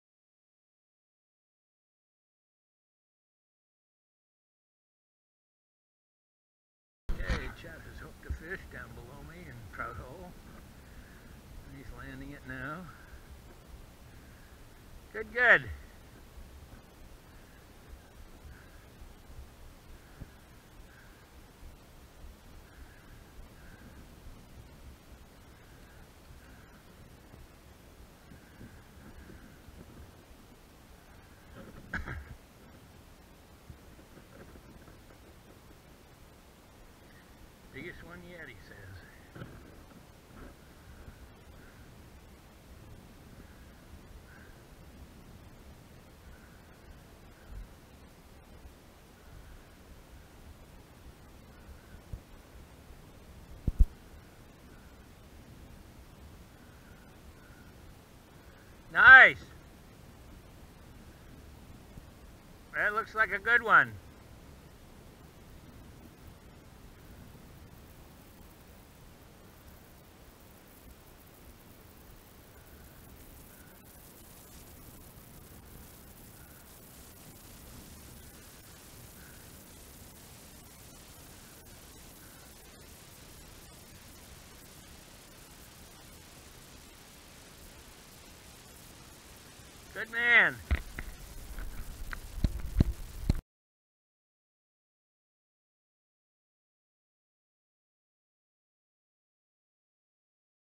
20200925 FBJ VIDEO #10 -- Chett gets his fifth fish casting in Trout Hole on 25 Sept 2020 at 11:08AM
In this short 1-1/2 minute video, Fred is reeling in his fly and Chett is landing his fifth fish of the trip down below where Fred has been fishing in Trout Hole. Fred waits until Chett has successfully landed his fish and holds it up, and Chett says that it’s his “biggest one yet” at about 18” in length. Then I shut off the camera and head back to the canoe to paddle down to where Chett is to pick him up and take us down a short ways to land just below where the grass ends so we can fish at the top end of Hog Hole where we have been seeing busting fish…